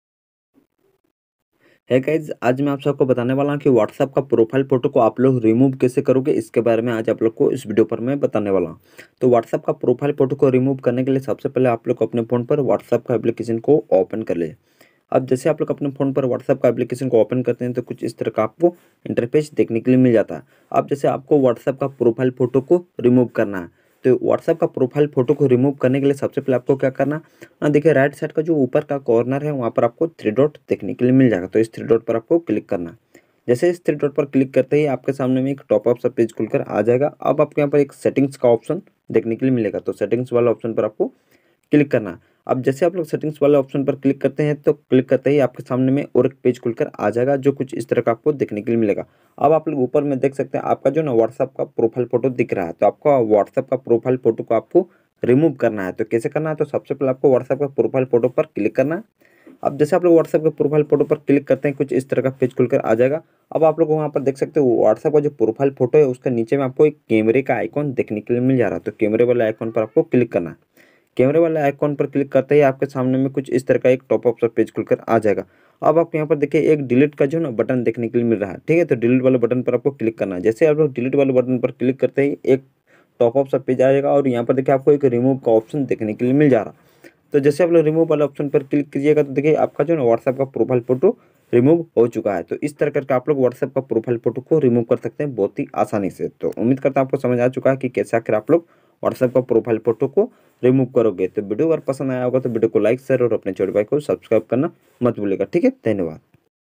WhatsApp ka profile photo remove Kaise kare||How to remove WhatsApp profile photo
Hi

आज मेने इस Video पर ये बताया हु कि आप WhatsApp ka profile photo remove कैसे करेंगे!